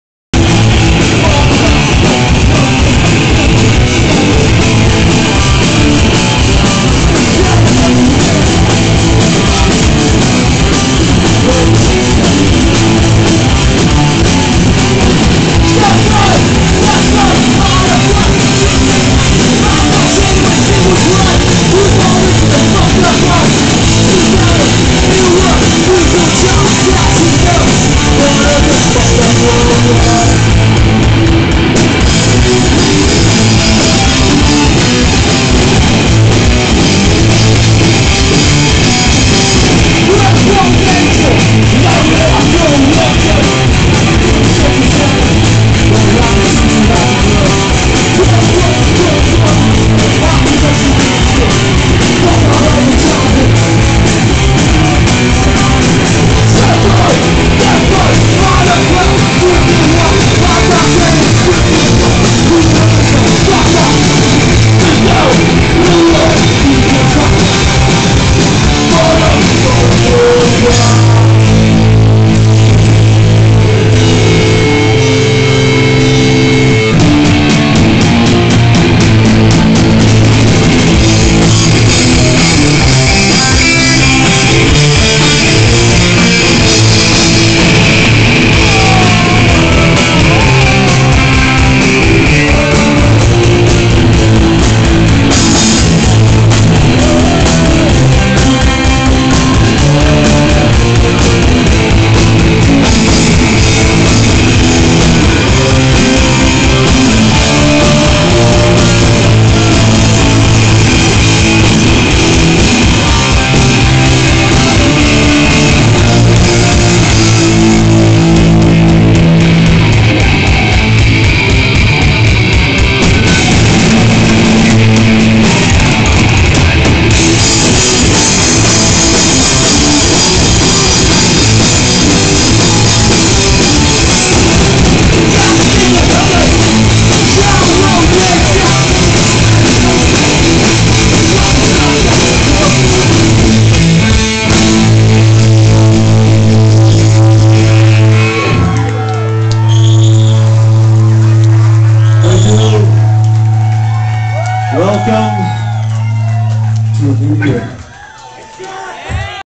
poison idea new years eve 2008 pt. 2
poison idea playing satyricon portland or new years eve 2008
this video is property of Brian Rodriguez and may not be used in any way with out my consultation before hand.